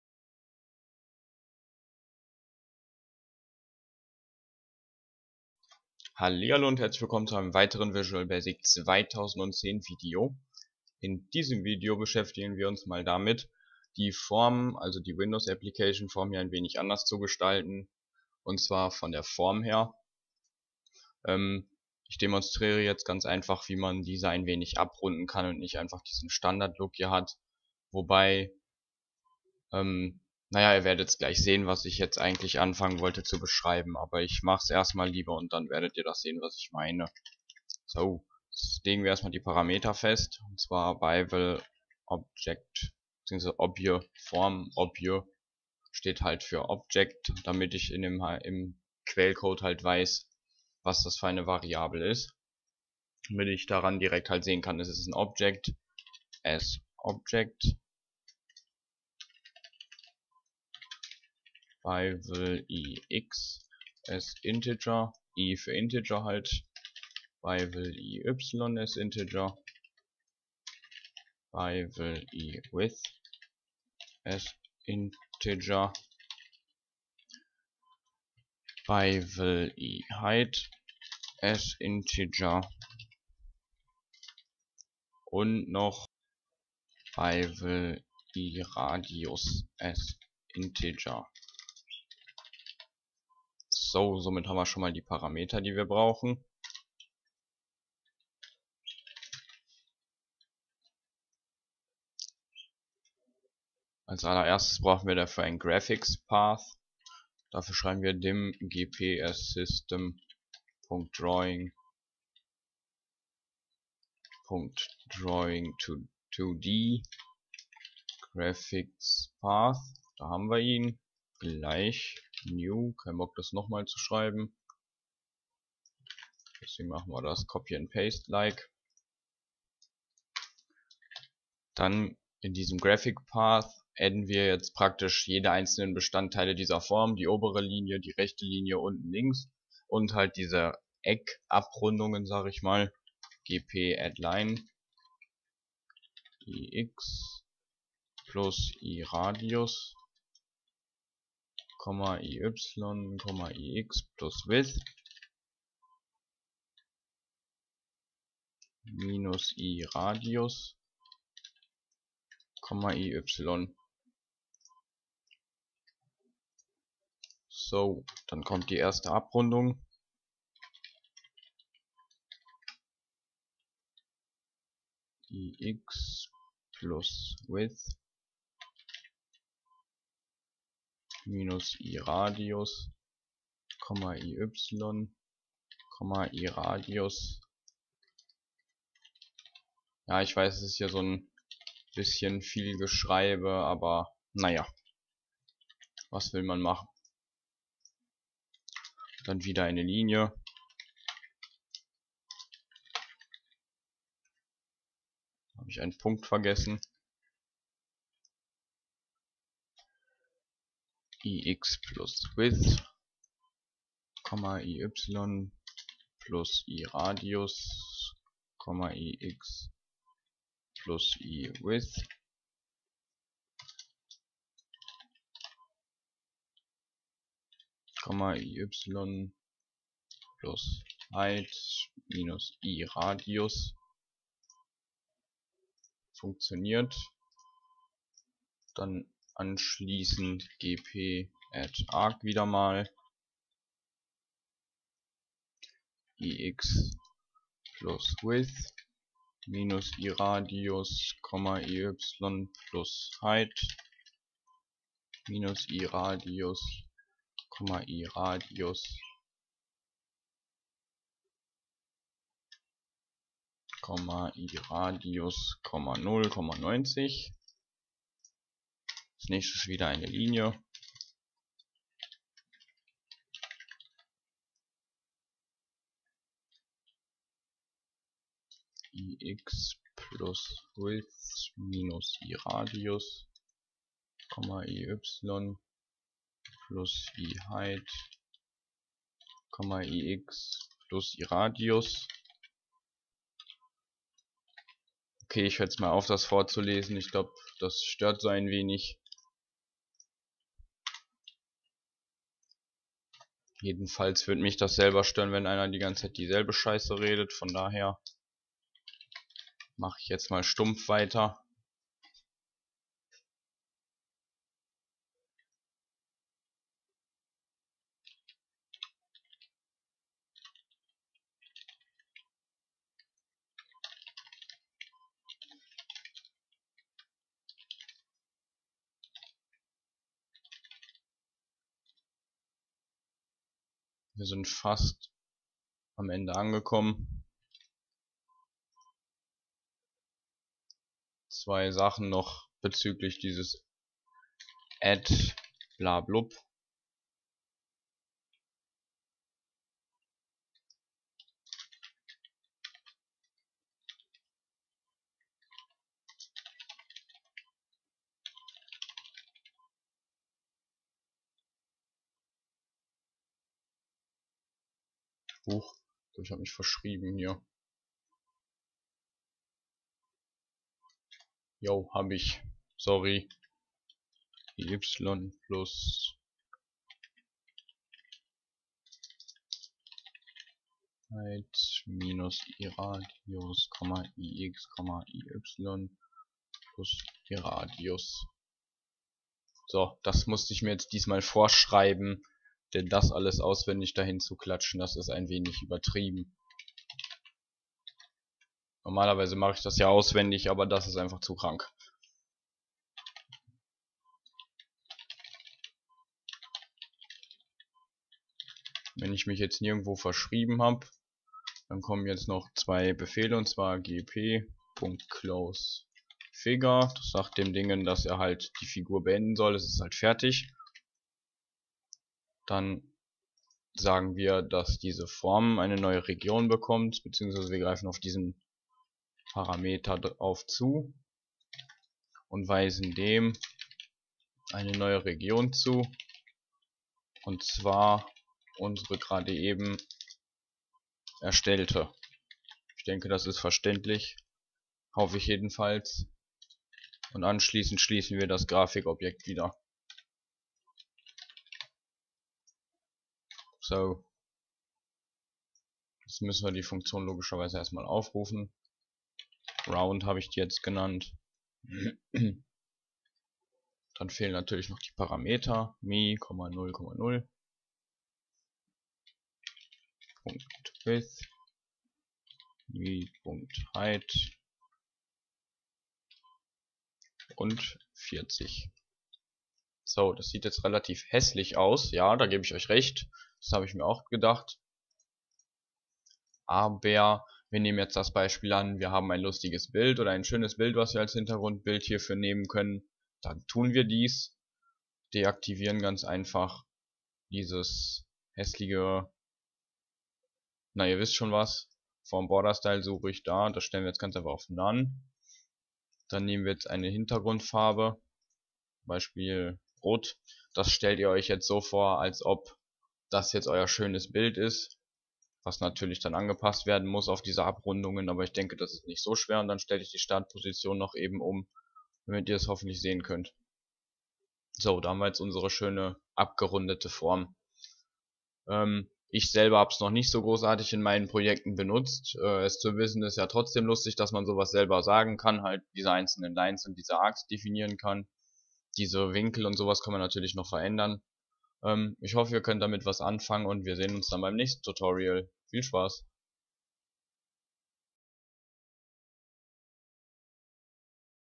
VB.NET Tutorial 47 - Form mit abgerundeten Ecken
In diesem Video erkläre ich euch, wie ihr eure Form ein wenig anders Designen könnt. Ich habe diese Art und Weise selber noch nicht oft in meinen Projekten benutzt, aber lernen ist ja wieder was anderes als benutzen "müssen". Zusätzlich denke ich aber, dass diese Methode bei zb. Gameclients ziemlich cool sein wird :).